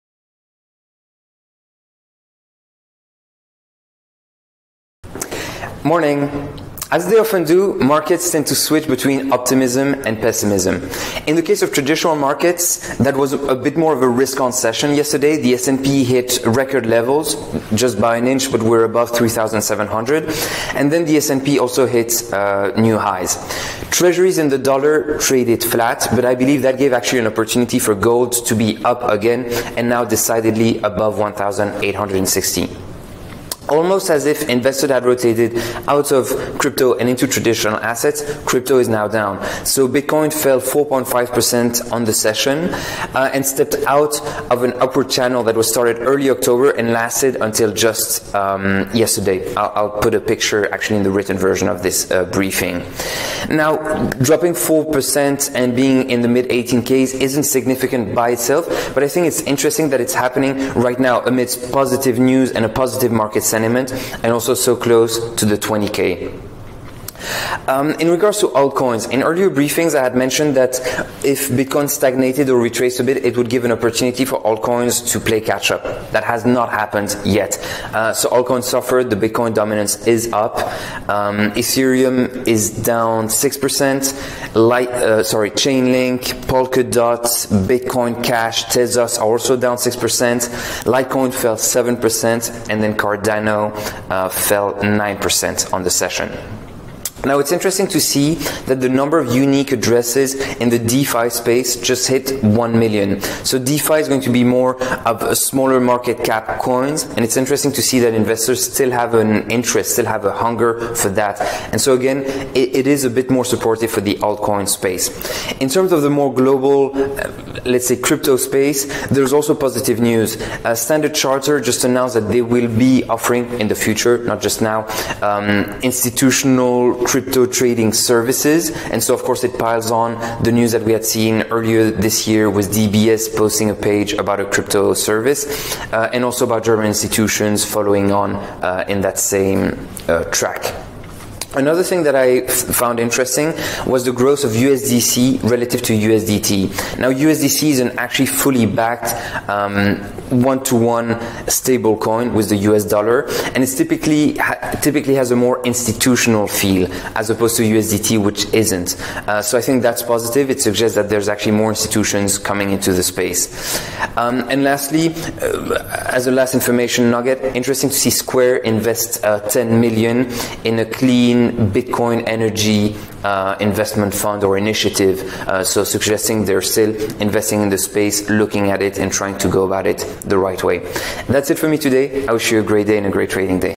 EQUOS Morning Update: December 9, 2020
As they often do, markets switch from pessimism to optimism and vice-versa.
In the case of traditional markets, yesterday was a bit more risk-on. The S&P closed at new highs, by barely an inch, but currently above $3,700. The Nasdaq also hit fresh record levels. Treasuries and the dollar traded muted. Then we saw gold, up again, now decidedly above $1,860.
Almost as if investors had rotated out of crypto and into traditional assets, crypto is down. BTC dropped about 4.5%, stepping out of the upward channel we saw from early October until yesterday. At the time of writing, the price is at $18,350. While 4% isn’t much and the mid 18K’s isn’t a low level by any means, the fall amid positive news and so close to the 20K mark it is worth noting and begets the question: “will traders exit now?”
Image for post
Alts are naturally not doing too well and the BTC Dominance index is up… but not by that much. In previous briefings, I alluded to the fact that a BTC stagnation could be an opportunity for smaller cap coins to play catch up. Let’s see if that happens.
For now, though, ETH is down 6% on the session, at $555. LINK, DOT, XTZ, and BCH followed down 6%. LTC fell 7%, and ADA is down about 9%.
It’s worth noting that the number of unique DeFi addresses rose above 1 million for the first time ever, suggesting that the interest in those types of token is definitely not dying out.
In regards to the broader crypto space, there is positive news: Standard Chartered is set to launch crypto trading capabilities for institutional investors, adding to the list of banks already doing so (DBS, German institutions, etc.).
It’s also worth noting that USDC is gaining traction over USDT. While that might sound trivial, a truly USD-backed stablecoin shows interest from what are probably more credible players, and an overall shift toward more reliable means of trading. This is very positive.
Lastly, did you see that Square invested $10 million in a Bitcoin Clean Energy Investment Initiative?